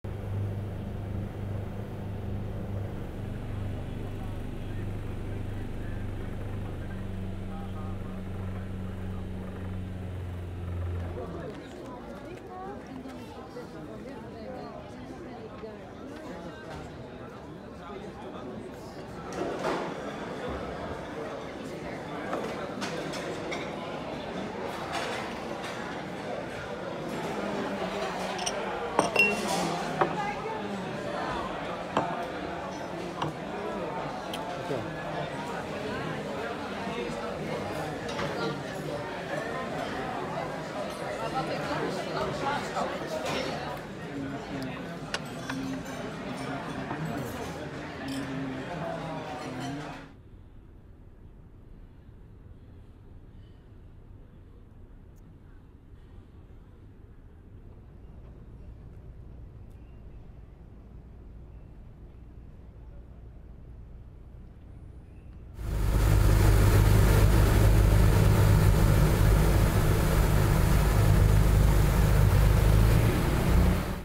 20181008 ~ Hotel New York
① memo 20181008 ~ Hotel New York ~ Diner at sunset last weekend in Hotel New York , set in the historic Holland Amerika Lijn building on the south bank of the Maas river in Rotterdam , Holland